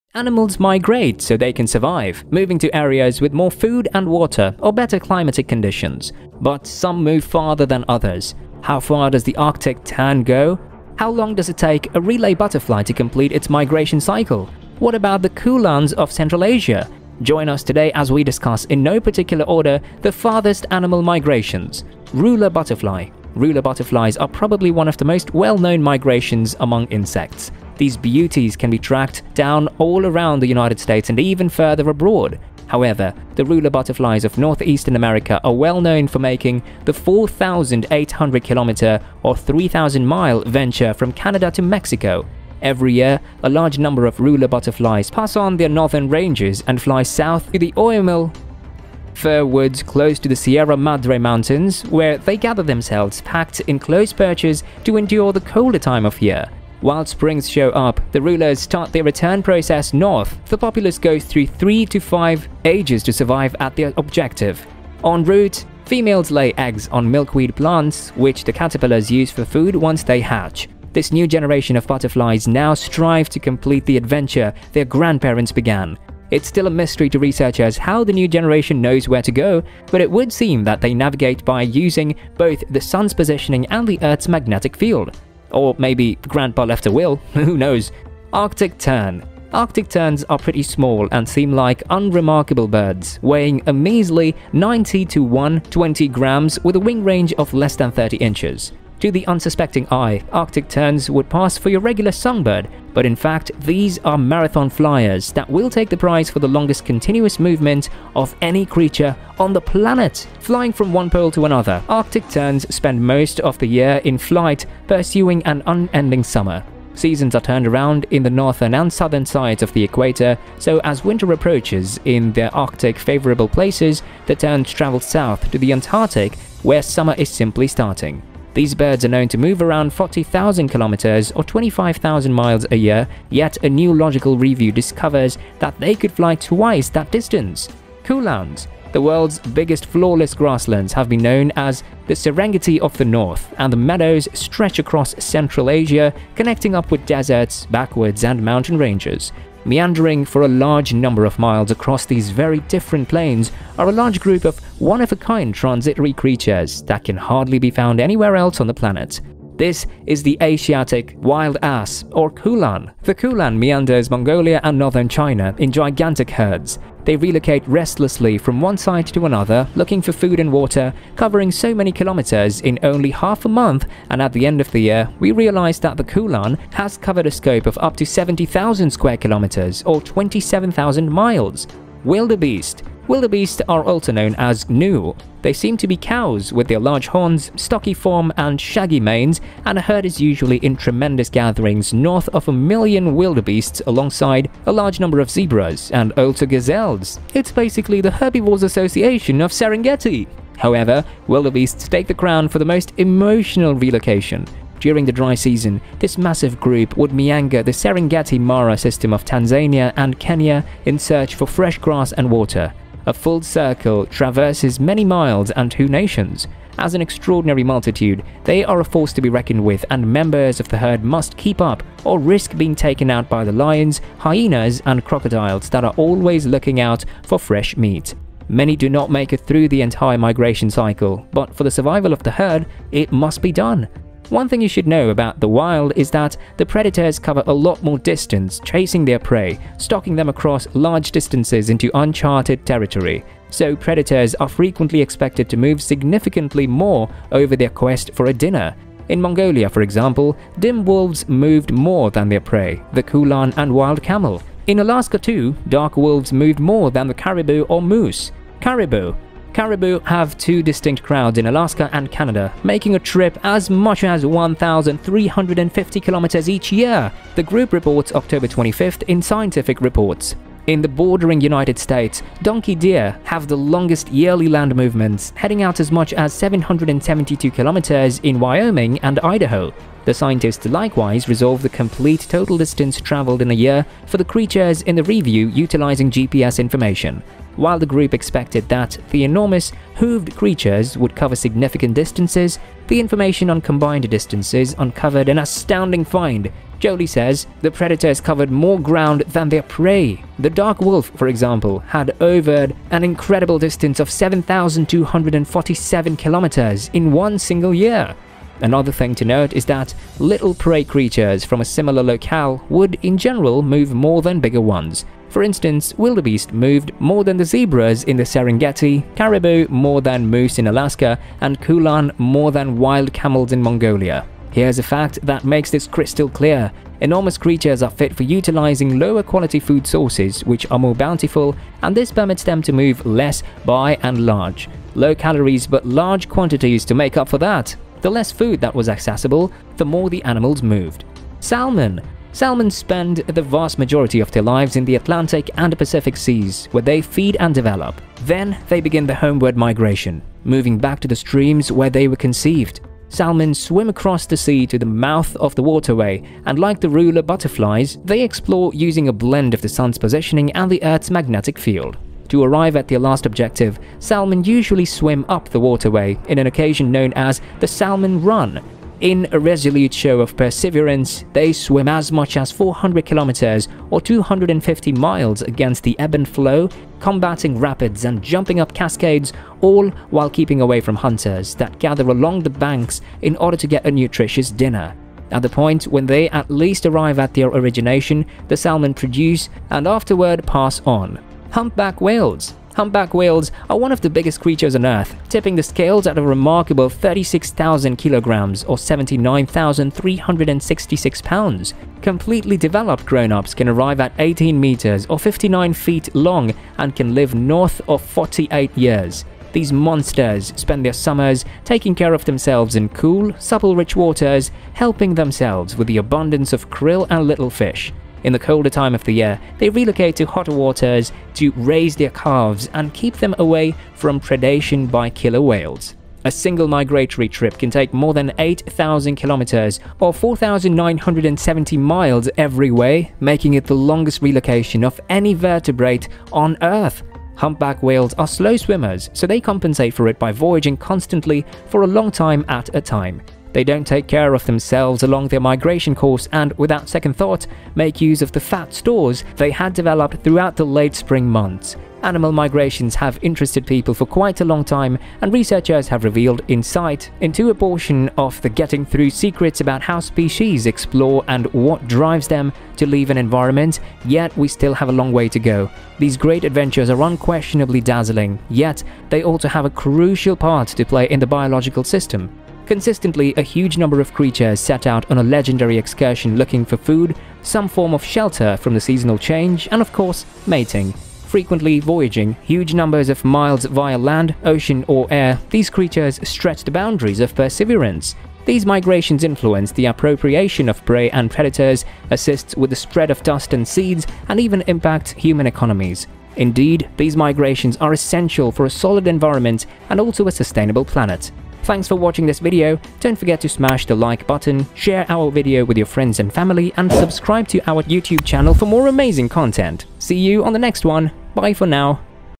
Caribou or Salmon: Who Travels the Furthest? Know About Animal Migration in Detail
In this video, we’ll be discussing animal migration in detail. Animals migrate, so they can survive, moving to areas with more food and water or better climatic conditions. But some move farther than others. How far does the Arctic tern go? How long does it take a relay butterfly to complete its migration cycle? What about the khulans of Central Asia?

Timestamps:

00:00 Intro
00:23 Ruler Butterfly
01:27 Arctic Terns
02:18 Khulans
03:02 Wildbeest
04:19 Caribou
04:33 Donkey Deer
05:42 Salmon
06:32 Humpback Whales

Join us today as we discuss in no particular order, the farthest animal migrations!

Make sure to watch this video on animal migration till the end so that you don't miss any updates on this mystery.